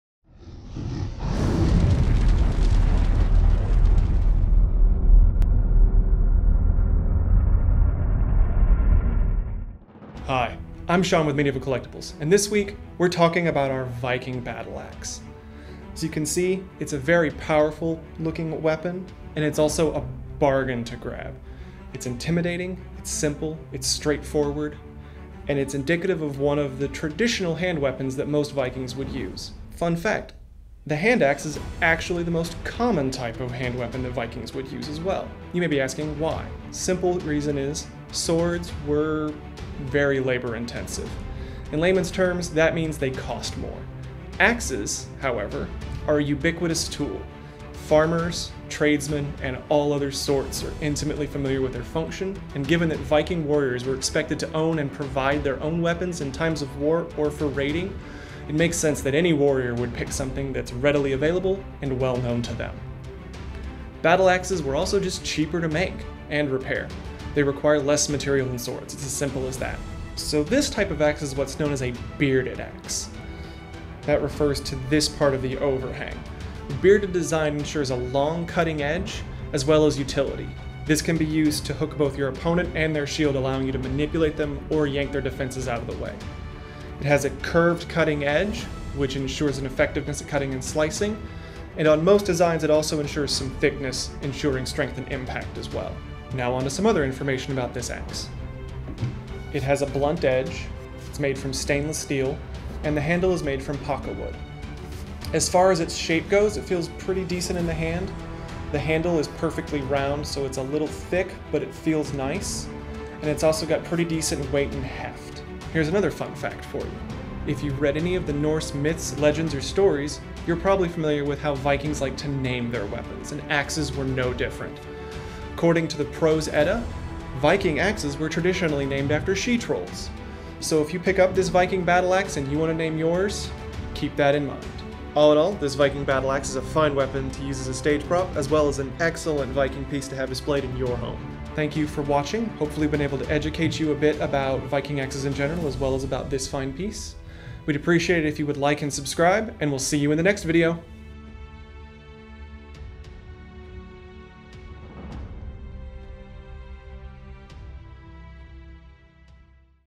ZS-882440 Viking Battle Axe from Medieval Collectibles
Take a seat and learn a bit about Vikings and their love for battle axes as Shawn explains the history and features of weapons like the Viking Battle Axe (ZS-882440) from Medieval Collectibles!
The Viking Battle Axe features a wooden handle that has a long stainless steel blade on the end. The traditional style and classic elegance creates the perfect setting for your medieval collection. The Viking Battle Axe has an overall length of 27 inches with a 12.5 inch long blade. The axe head has an extended beard, offering a dramatically wide cutting edge. This Viking Battle Axe makes a great display piece in your home, or it can be used as a realistic stage prop.